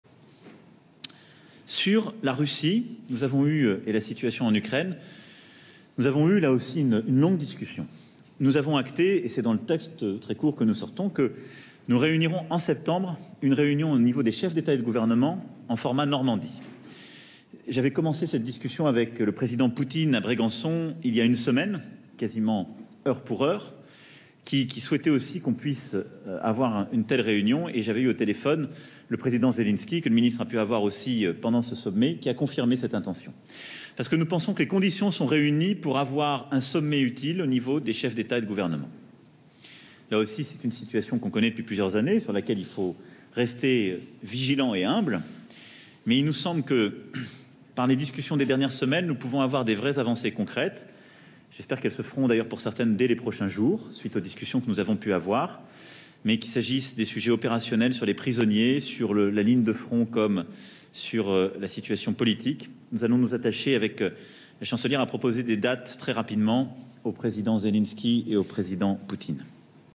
E. Macron : «Sur l'Ukraine, nous réunirons en septembre les chefs d’États et de gouvernement»
Emmanuel Macron a donné sa conférence de presse de clôture du sommet du G7. Le président français a annoncé qu'un sommet réunissant les dirigeants ukrainien, russe, allemand et français aurait lieu en septembre pour avancer vers la paix en Ukraine.